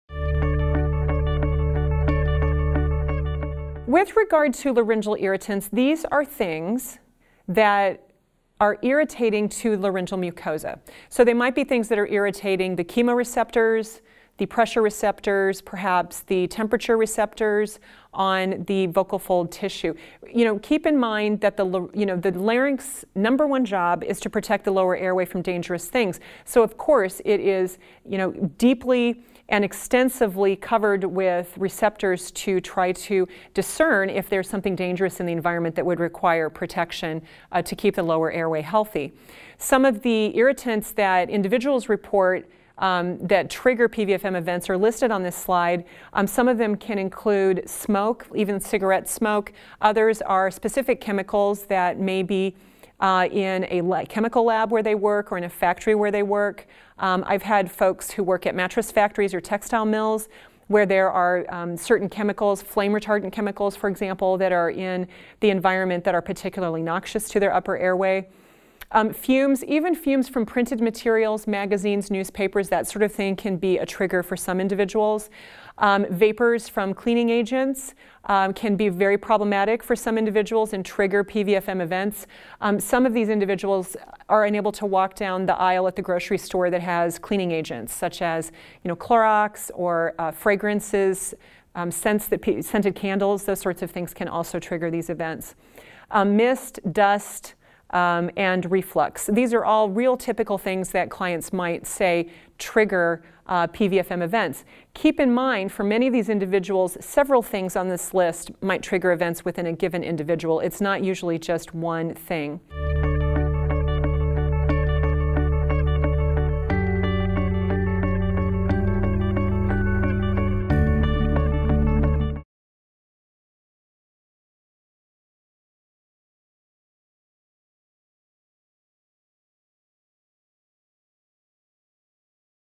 Laryngeal Irritants That May Trigger PVFM - Mary Sandage | MedBridge
Read related article: "Ruling Out Paradoxical Vocal Fold Motion/Vocal Cord Dysfunction Lookalikes"